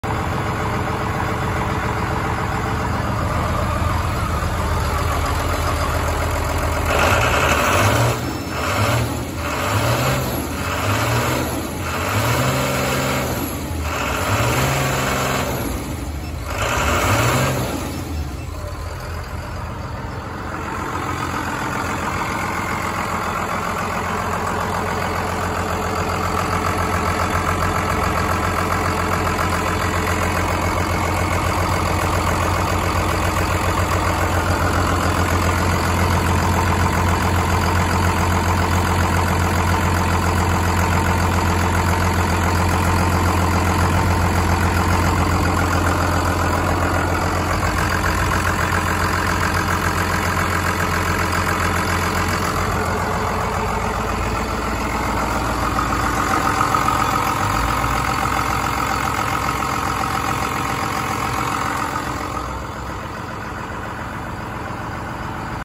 (P-32217) 2001 Cummins ISB Engine
(Used) 2001 Cummins ISB, ENGINE ASSEMBLY-
Displacement: 5.9L,
Horsepower: 185,
CPL: 2687,
Serial Number: 60280844,
Part Number: 4018419,
Model: DR1581-RX,